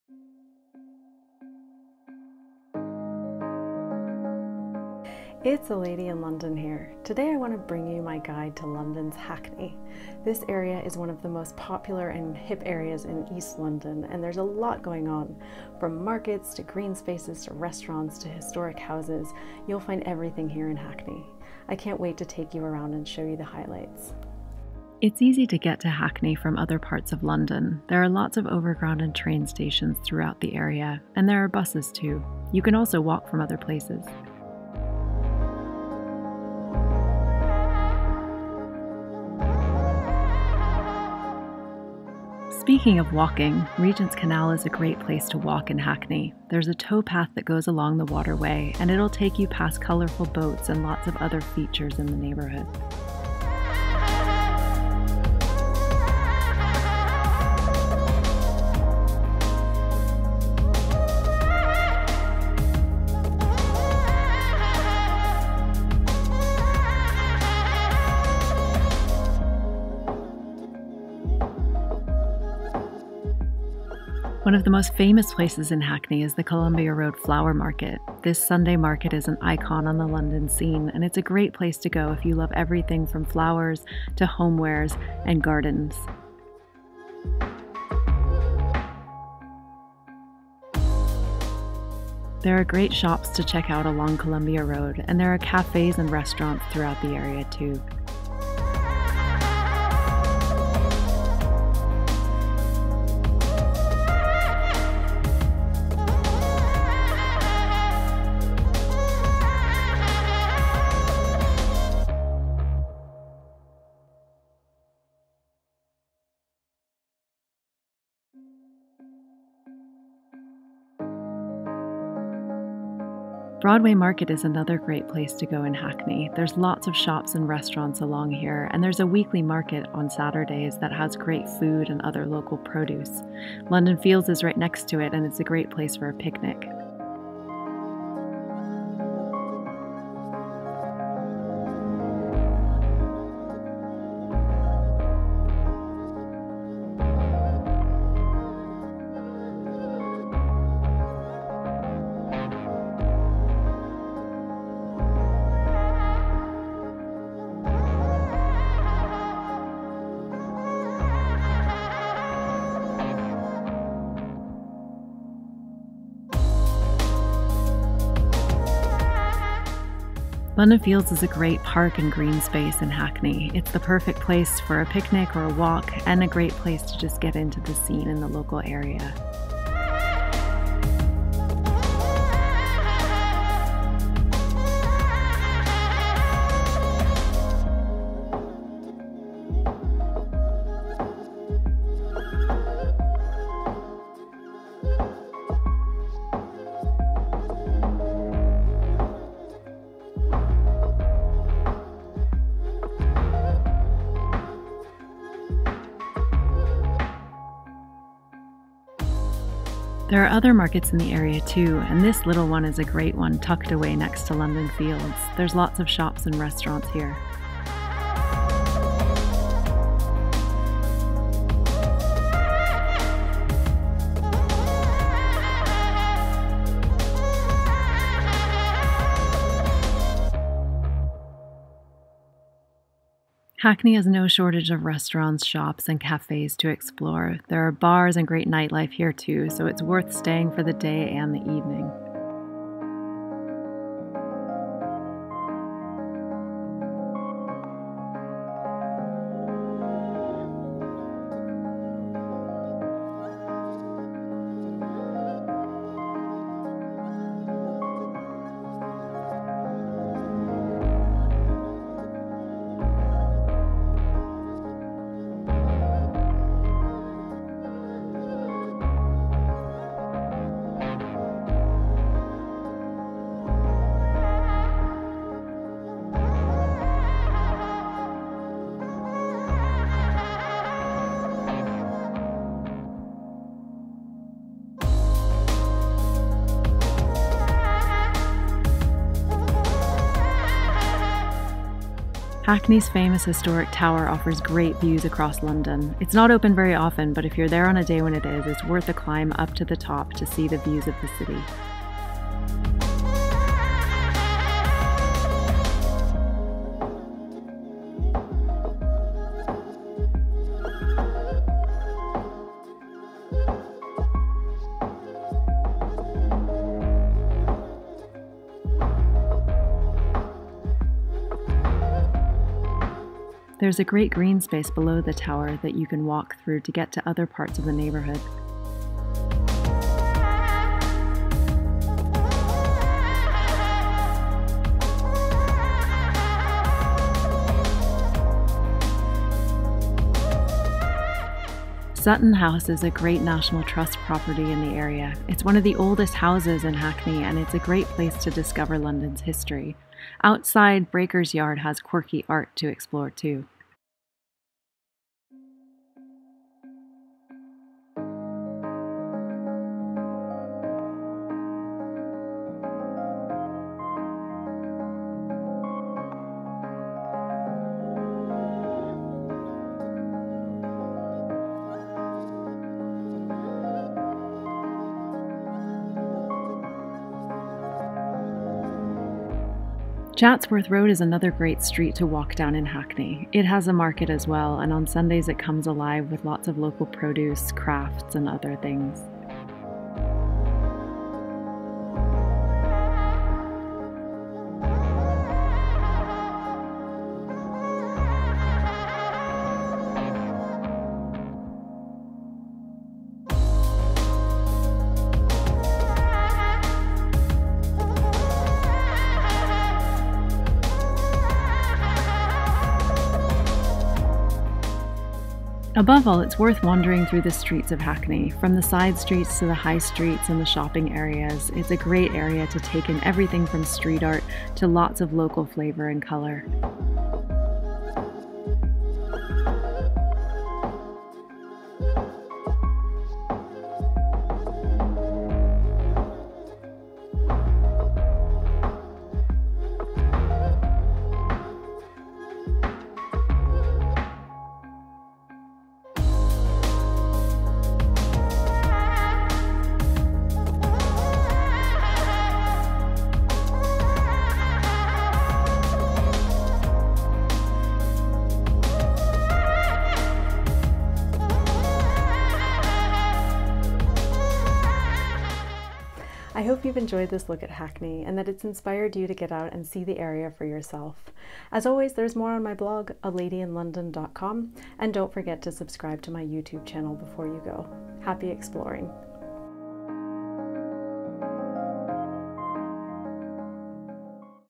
10 THINGS TO DO IN HACKNEY, LONDON | London Fields | Columbia Road Flower Market | Hackney Cafes
You can see more things to do in Hackney on the A Lady in London blog

This video is about 10 things to do in Hackney, London. It includes everything from the Columbia Road Flower Market to London Fields, Broadway Market, Regent's Canal, and Sutton House. From restaurants and bars to cafes and pubs, this part of east London has it all.